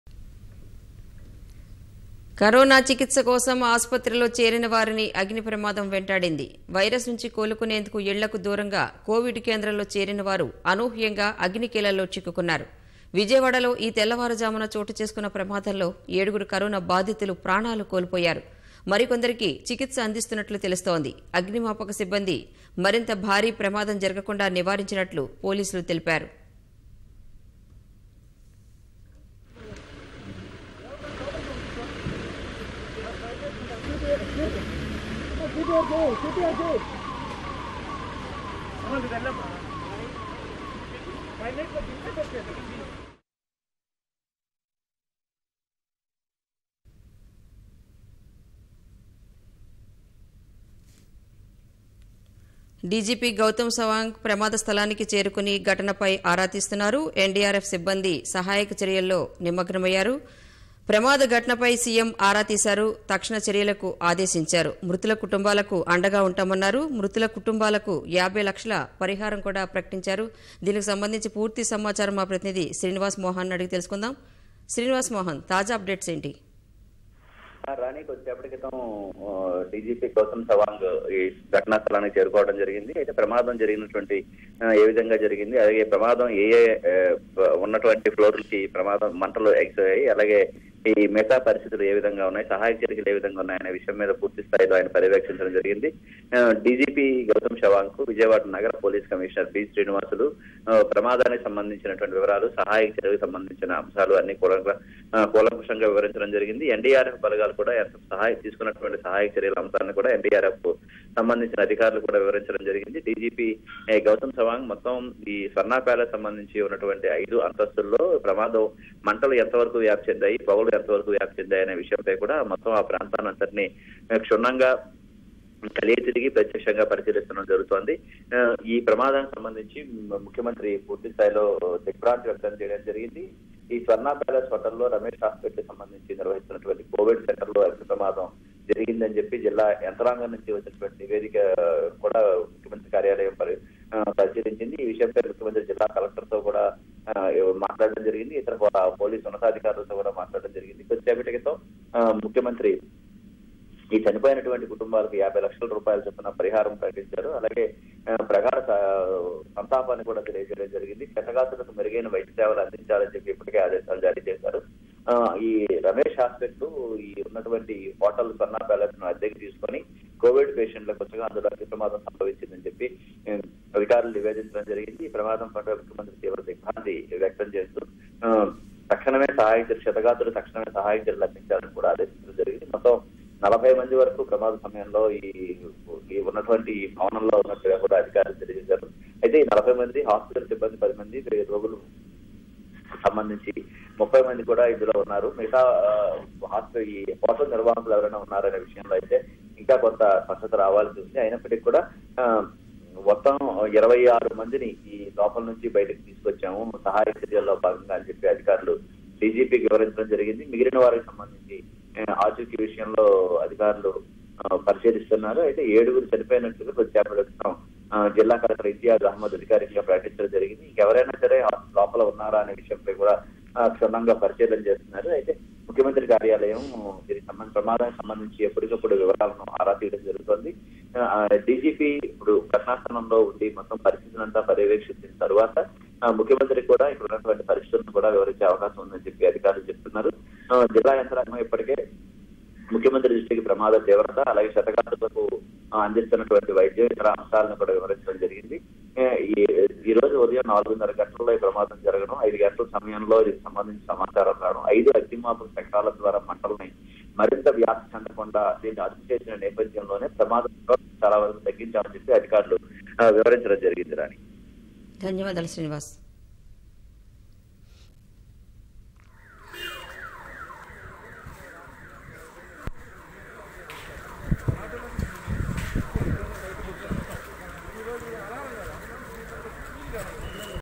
AP Govt Announces Rs.50 lakh Ex-Gratia to Victim Families | After Fire Accident
AP Govt Announces Rs. 50 lakh Ex - Gratia to Family of Killed in Fire Accident @ Covid Center | in Vijayawada